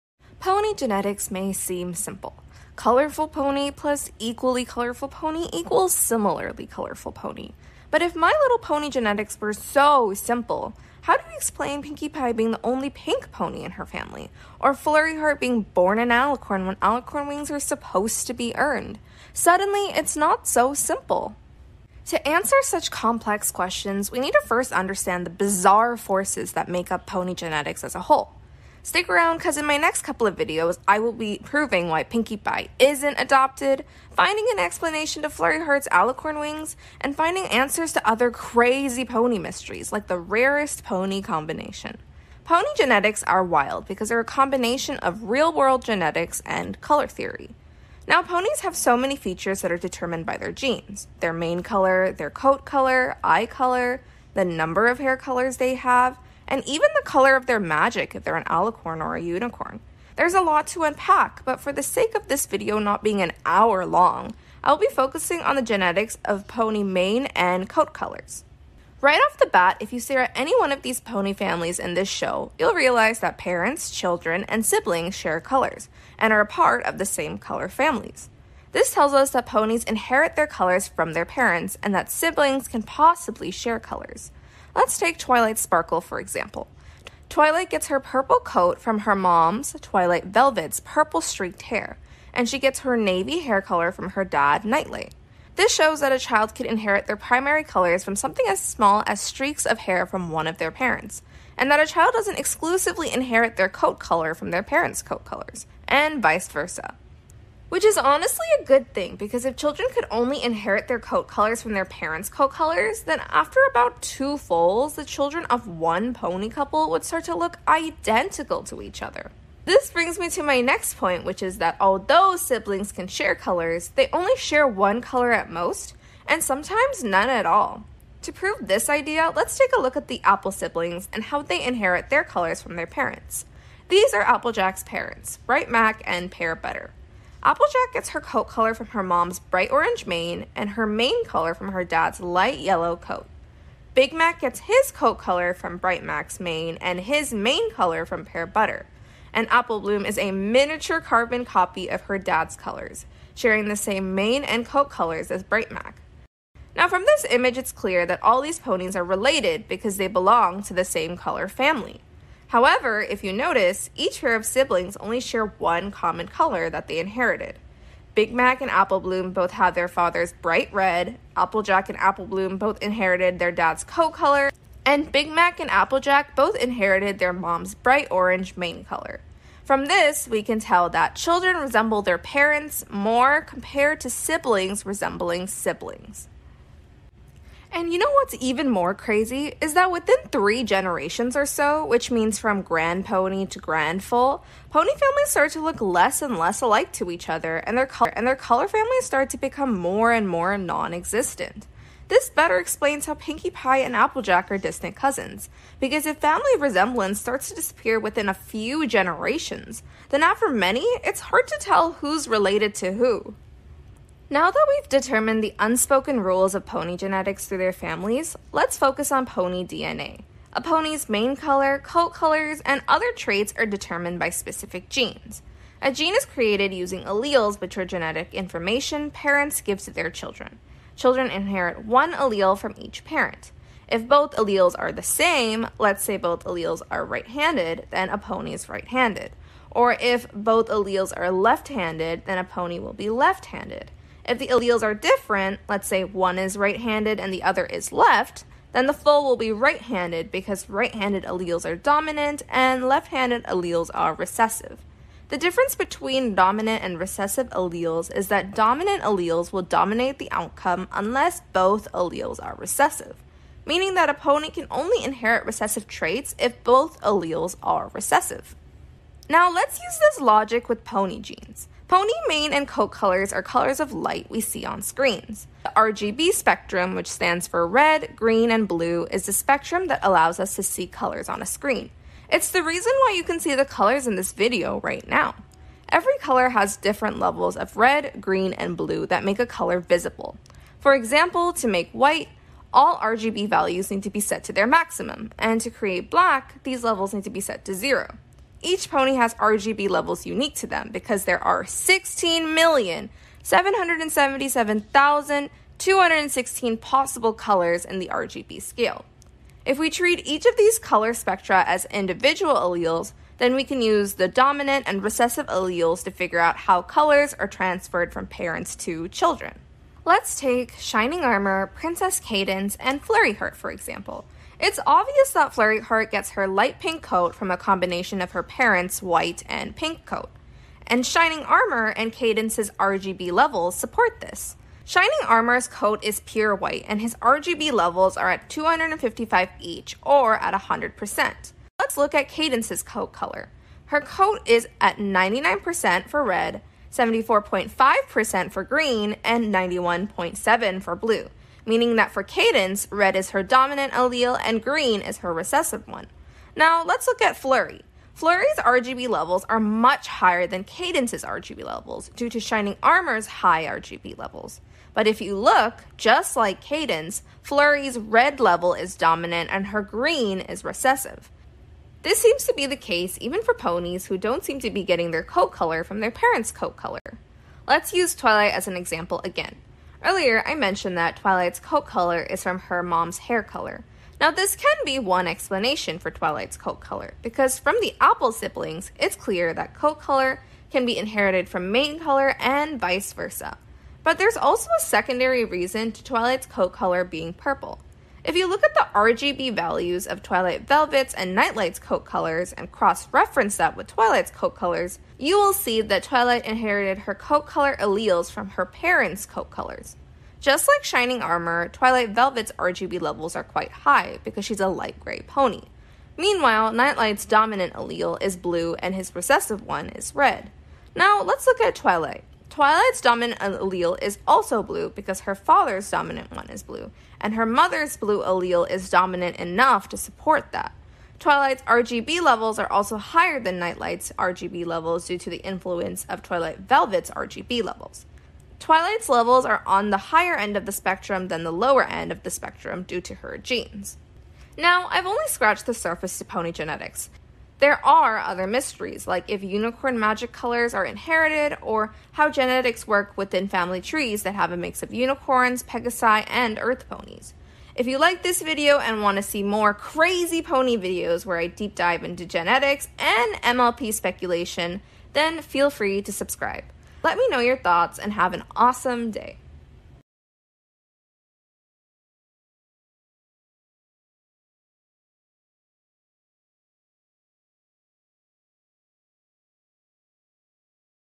I FIGURED OUT My Little Pony GENETICS (MLP Genetics)
Finally Figured out the FORMULA  to my little pony genetics (subtitles available :)

📌 timestamps
00:00 Intro
00:44 Basic Pony Genetics
01:27 Parents THEORY
01:57 Sibling THEORY
03:05 Generations THEORY
03:40 Genes & Alleles (Genetics)
04:34 Ponies RGB Spectrum
07:20 Outro